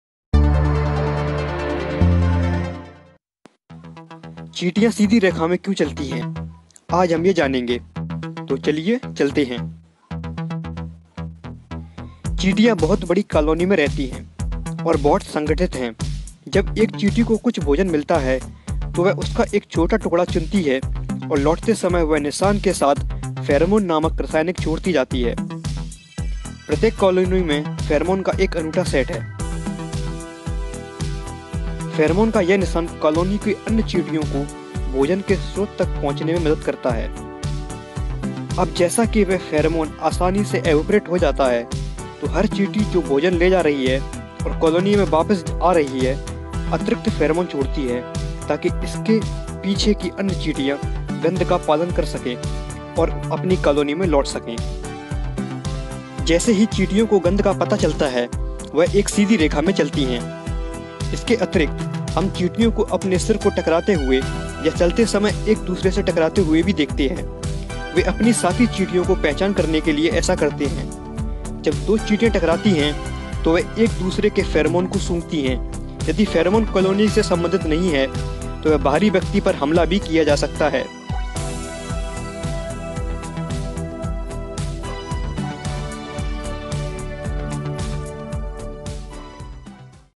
चीटी सीधी रेखा में क्यों चलती है?/Why do ants walk in a straight line? |HINDI
Ants live in big colonies and are very organized. When an ant finds some food, it picks a small piece of it and while returning, it leaves chemical scents called pheromones along the trail. Each colony has a unique set of pheromones. This trail of pheromones helps other ants of the colony to reach the source of food.

 Now, as these pheromones evaporate easily, every ant which is carrying food and returning to the colony leaves additional pheromones so that other ants behind it can follow the smell and return to their colony. As the ants follow the smell, they tend to walk in a straight line.

 In addition to this, we even see ants bumping their heads or colliding with each other while walking. They do this to identify their fellow ants. When two ants collide, they smell each other's pheromones. If the pheromones don't belong to the colony, the outsider ant might even be attacked.